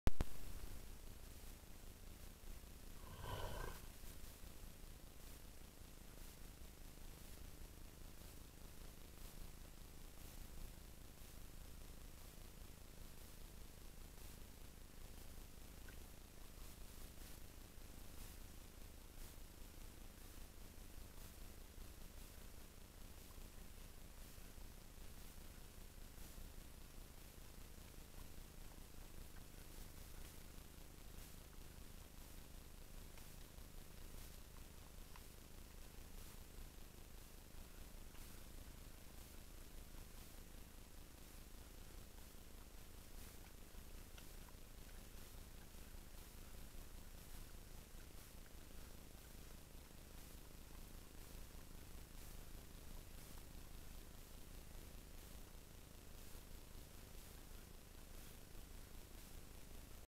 Лось совсем не в зимнем лесу
В этом году совсем не обычная зима. В место снега, грязь. Снег если и вы подает, то в течении нескольких дней весь таит. Наверное для животных это и не плохо.